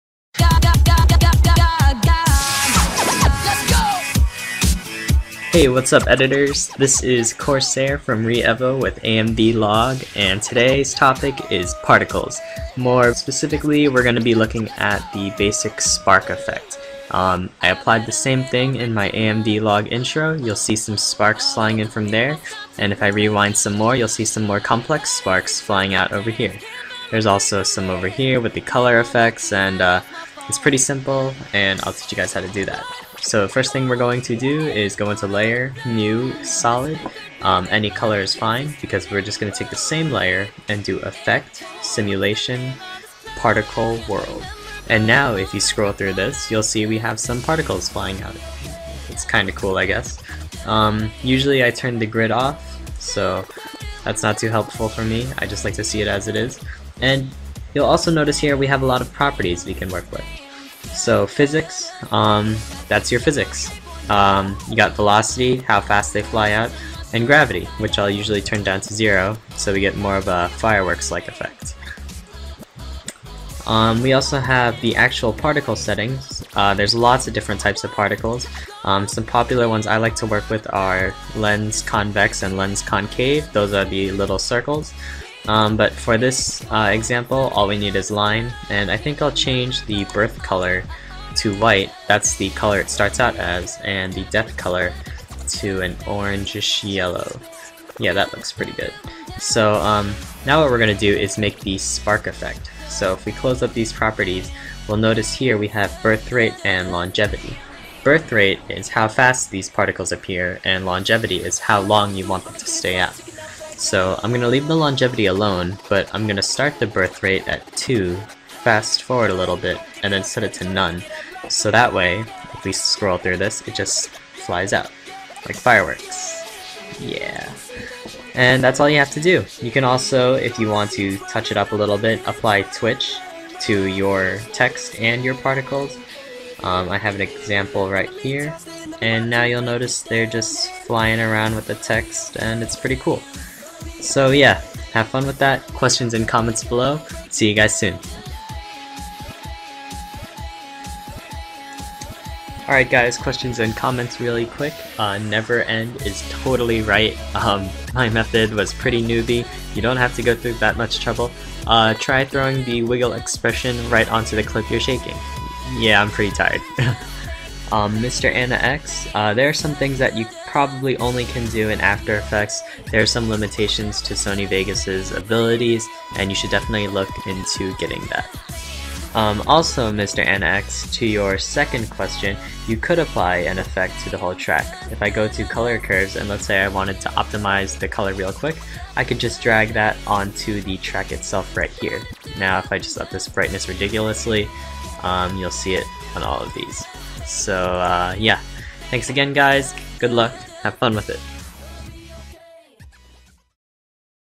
[AMVlog] Particles
Learn how to create a fireworks / spark effect like the one in the AMVlog intro!

Software - Adobe After Effects CS5